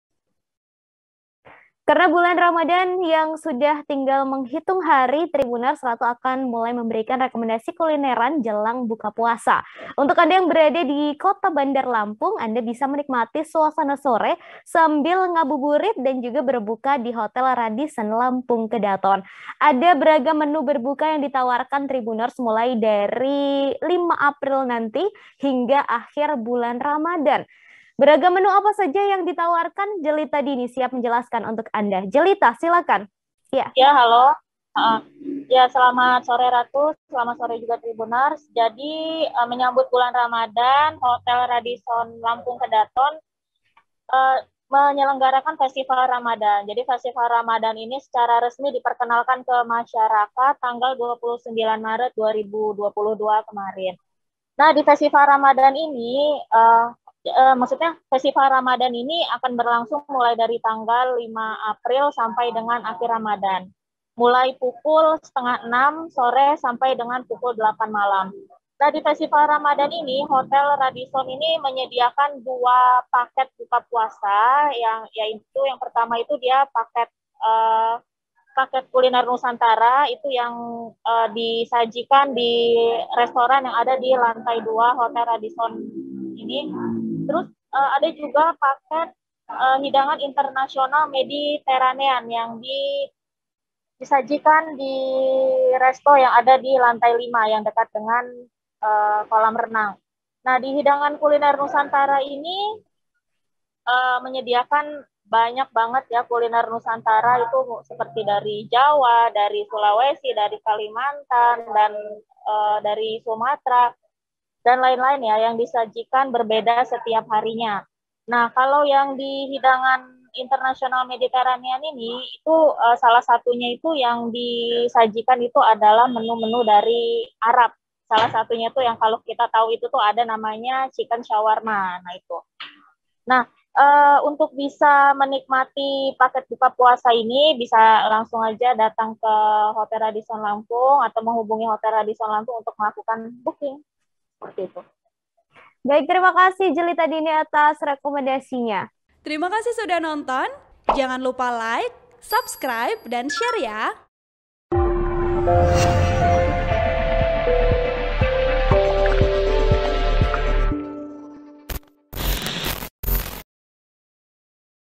Menu Spesial Ramadan di Hotel Radisson Lampung, Makanan dari Berbagai Negara Spesial untuk Berbuka
TRIBUN-VIDEO.COM - muslim khususnya di Kota Bandar Lampung bersiap untuk berburu kuliner buka puasa.

Di Hotel Radisson Lampung Kedaton, ada tawaran menu menu menarik untuk berbuka puasa.

Mulai 5 April hingga akhir Ramadan, silakan datang ke Festival Ramadan di Hotel Radisson untuk memilih dua paket buka puasa, yakni kuliner nusantara dan kuliner internasional mediterania.

Harganya cukup menarik, tamu bisa menikmati sepuasnya seluruh menu dalam paket hidangan kuliner nusantara dengan tarif Rp 185 ribu per orang.

Sementara untuk paket hidangan kuliner internasional, tarifnya Rp 265 ribu per orang.

(Tribun-Video.com)